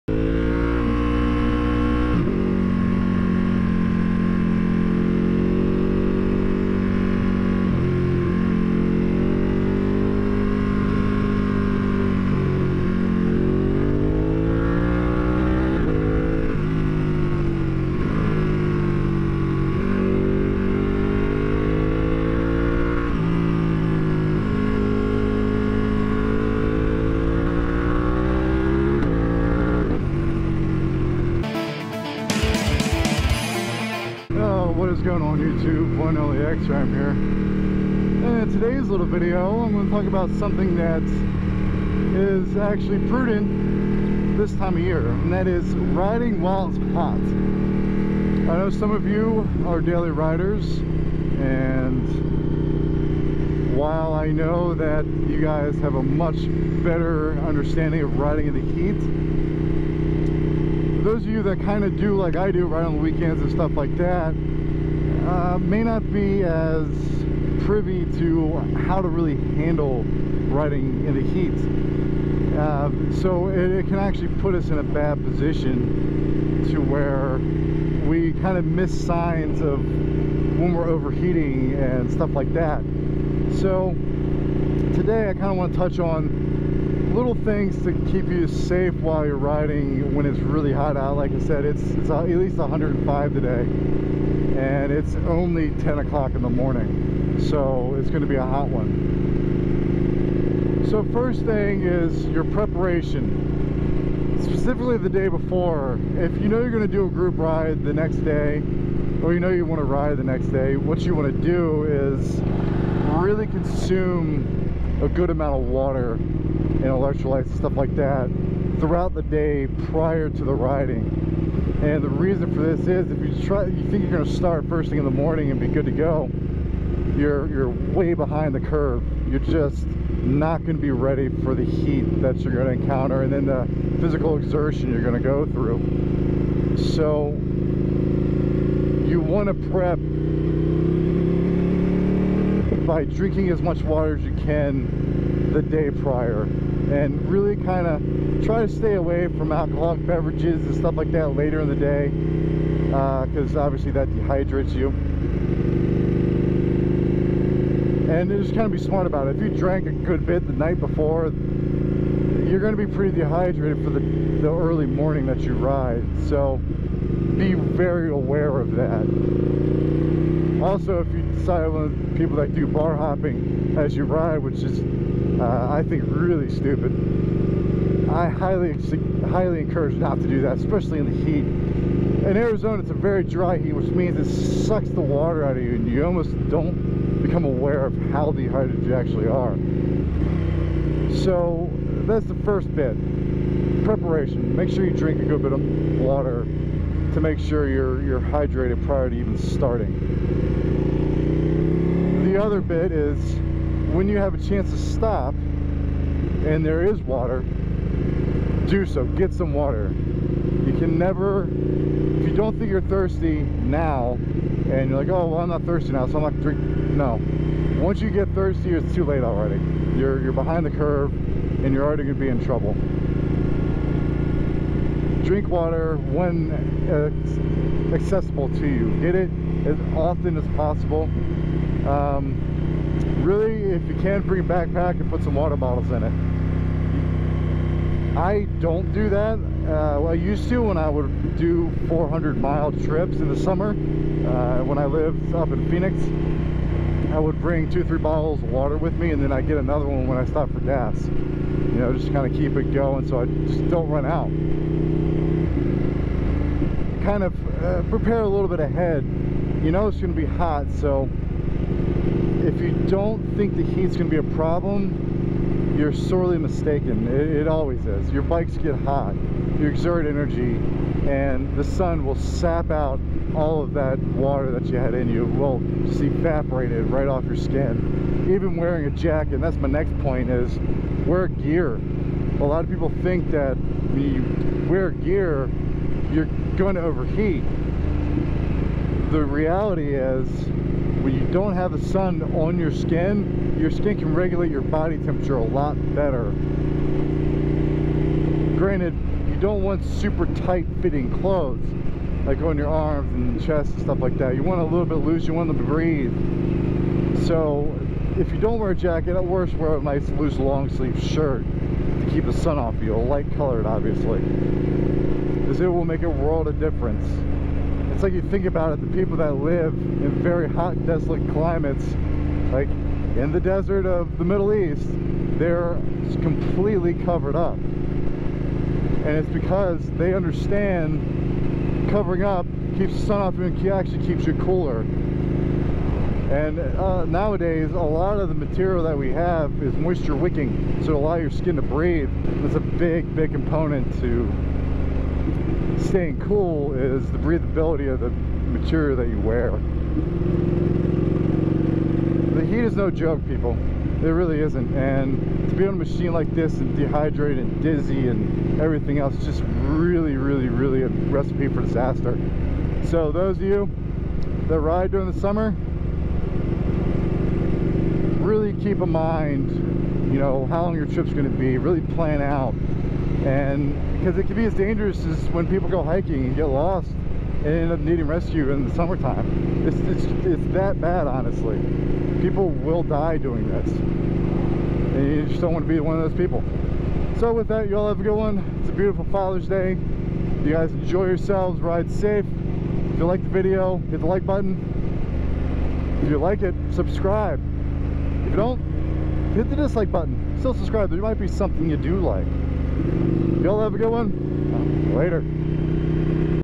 How to ride in the heat!
This video I go over steps to take when riding in the heat. If you aren't an everyday rider and you like to take your motorcycle out on the weekends, you might be missing some key things to keep yourself safe.

Arizona gets rather hot and not being prepared for the toll heat can take on your body is a dangerous situation.